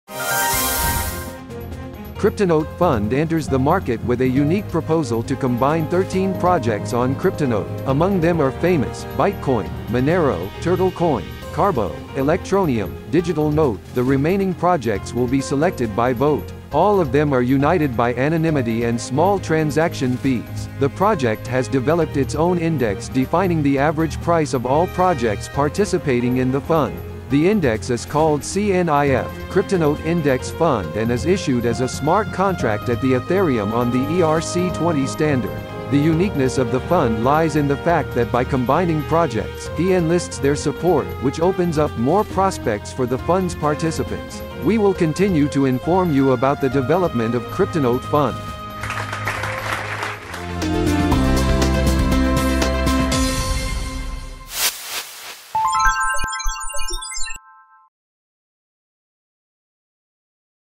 KCN: Trade the Index Token from CryptoNote
cryptonote.fund enters the market with a unique proposal to combine 13 projects on cryptonote. Among them are famous: Bytecoin, Monero, Turtlecoin, Karbo, Electroneum, Digital note, the remaining projects will be selected by vote. All of them are united by anonymity and small transaction fees. The project has developed its own index defining the average price of all projects participating in the fund. The index is called CNIF - CryptoNote Index Fund and is issued as a smart contract at the Ethereum on the ERC20 standard. The uniqueness of the fund lies in the fact that by combining projects, he enlists their support, which opens up more prospects for the fund's participants. We will continue to inform you about the development of cryptonote.fund